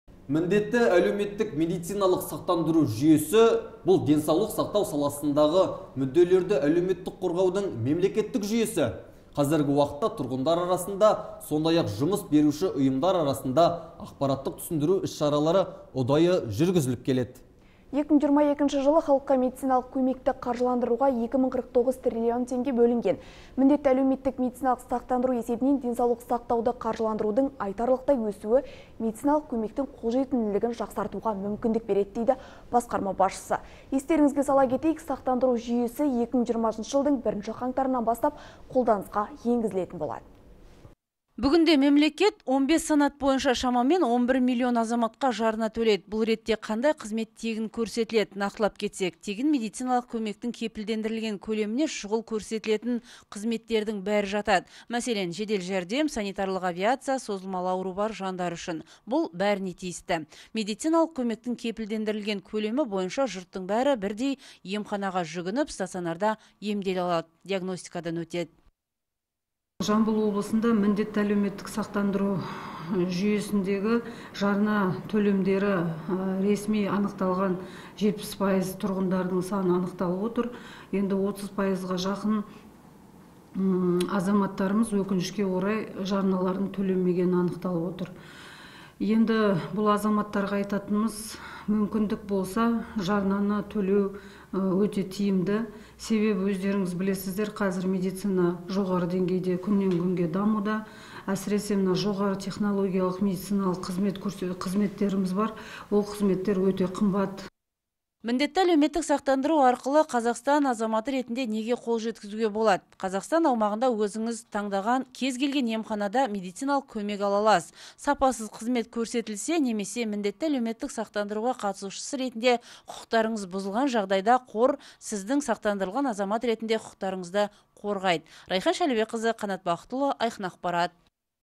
Міндетті әлеуметтік сақтандырудың міндеті қандай?
Міндетті әлеуметтік медициналық сақтандыру жүйесі – бұл денсаулық сақтау саласындағы мүдделерді әлеуметтік қорғаудың мемлекеттік жүйесі. Қазіргі уақытта тұрғындар арасында, сондай-ақ жұмыс беруші ұйымдар арасында ақпараттық түсіндіру іс-шаралары ұдайы жүрнгізілірп келеді.

2022 жылы халыққа медициналық көмекті қаржыландыруға 2,049 трлн теңге бөлінген. МӘМС есебінен денсаулық сақтауды қаржыландырудың айтарлықтай өсуі медициналық көмектің қолжетімділігін жақсартуға мүмкіндік берді дейді, басқарма басшысы. Естеріңізге сала кетейік сақтандыру жүйесі 2020 жылдың бірінші қаңтарынан бастан қолданысқа енгізілген болатын.